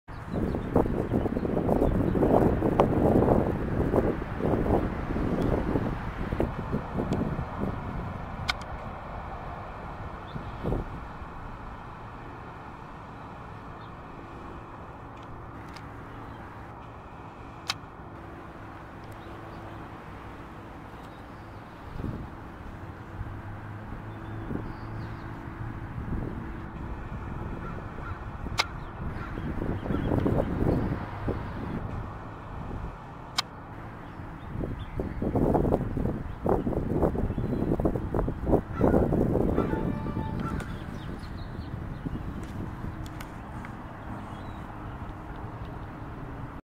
( DSCN4598 ) Geese Snacking Spot ~ Bobby Hutton Park
( OCT3 / 2019 ) Oakland, California

@Bobby Hutton Park ( Defremery Park ) / West Oakland